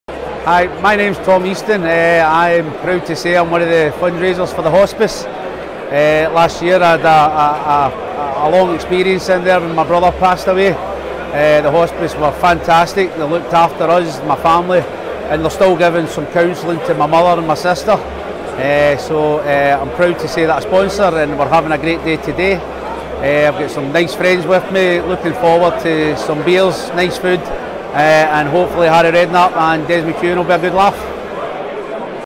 Tom Eason | A Hospice Supporter
A personal message from Tom who has a personal connection with the Glasgow Hospice.